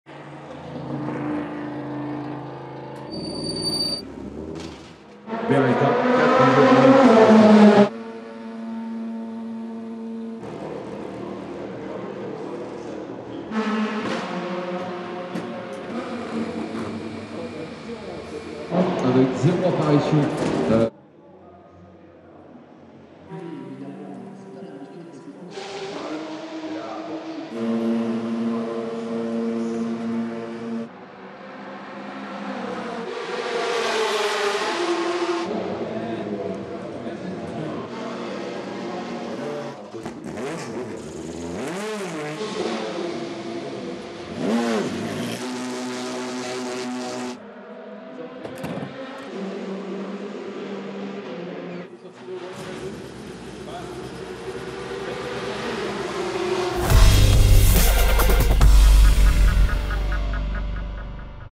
Le Mans 2014 - ambiance dans les stands lors de la première séance d'essais du mercredi 11 juin
Si sur la piste, les pilotes affolent les chronos, dans les stands l'ambiance est studieuse et le balai des ingénieurs et mécaniciens s'organise. Retrouvez l'atmosphère de la Pit-Lane, lors de la première séance d'essais du mercredi 11 juin.